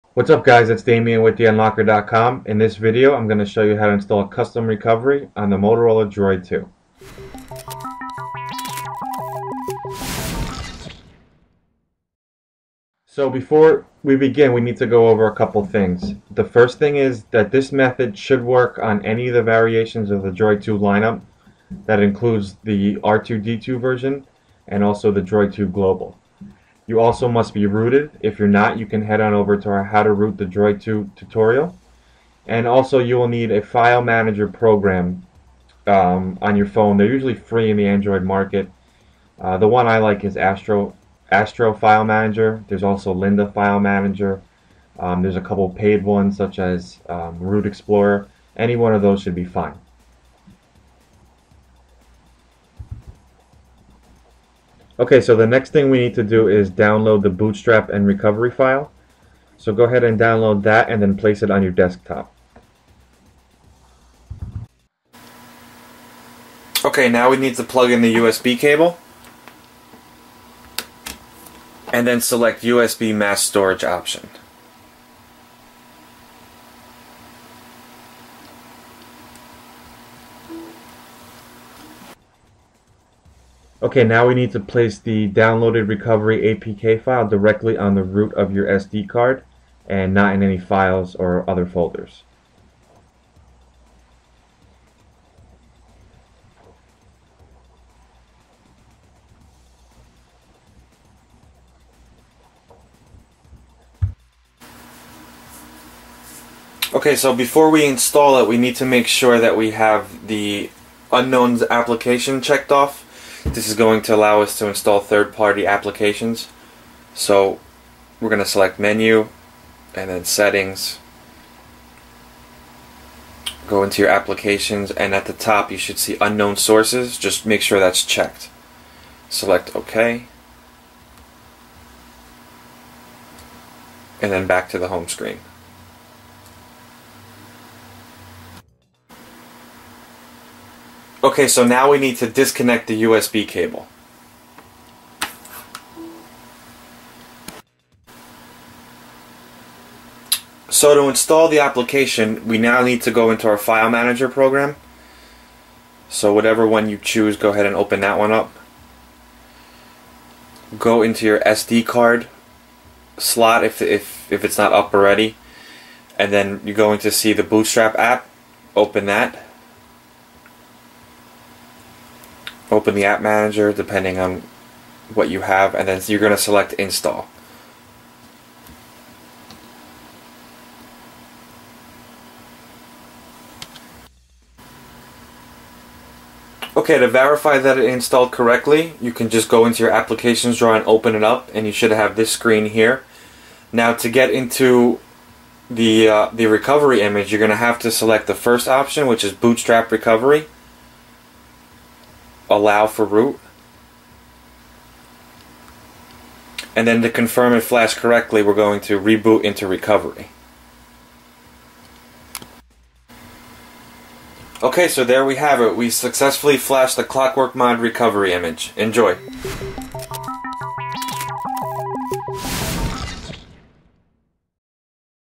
How To Load a Custom Recovery Image on the Motorola Droid 2
For the necessary files head to our site above.

Do you have a rooted Droid 2 and want to install a custom ROM? The most important step is having a custom recovery image